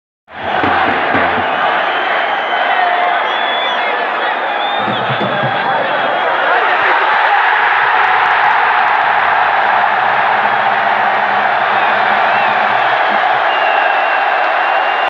Golul lui Pintilii in Steaua-Viitorul 1-1 vazut din spatele portii (2017), by Cristi Otopeanu
VIDEO: Cristian Otopeanu

Golul lui Mihai Pintilii in Steaua-Viitorul 1-1, vazut din spatele portii (2017)

FCSB / Steaua a remizat cu FC Viitorul, in etapa a saptea a play-off-ului Ligii I, scor 1-1.

Au marcat: Pintilii '40 pentru gazde şi Tucudean '73 pentru oaspeţi

FCSB / Steaua: Nita - Pleasca (Tamas '81), Balasa, Moke, Momcilovici - Pintilii, Ovidiu Popescu (Gnohere '76) - Enache (De Amorim '60), Boldrin, Tanase - Alibec. Antrenor: Laurentiu Reghecampf

FC Viitorul: Ramniceanu - Hodorogea, Taru, Boli, Cristi Ganea - Benzar, Rosu, Vana - Purece (Casap '55), Chitu (Tucudean '70), Florinel Coman (Morar '70). Antrenor: Gheorghe Hagi